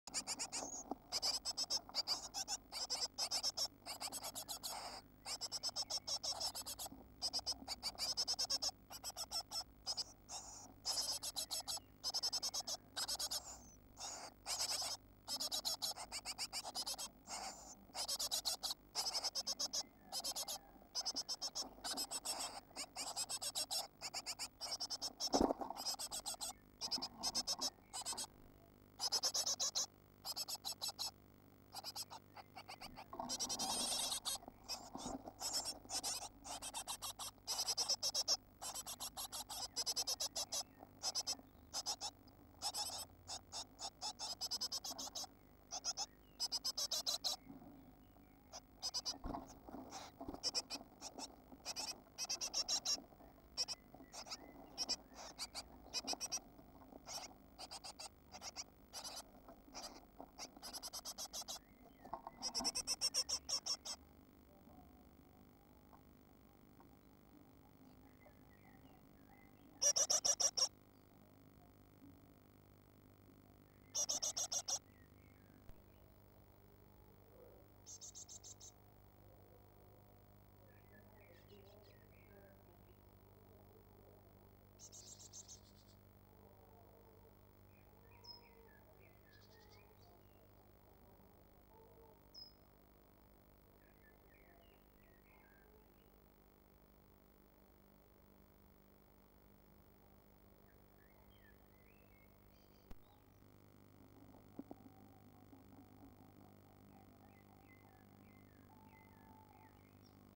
Teasing the chiks with caterpillar, trying to make them fled
Teasing the chiks with caterpillar, trying to make them fledge // Naar buiten lokken met rups - Captured Live on Ustream